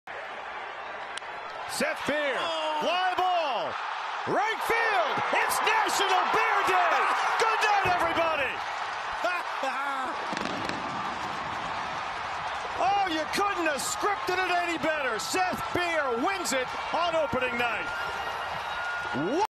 SETH BEER with first walk-off of the 2022 Season! 🔥
A MOONSHOT by Beer. And it’s national beer day. Coincidence?

All clips used belong respectively to the MLB, not me.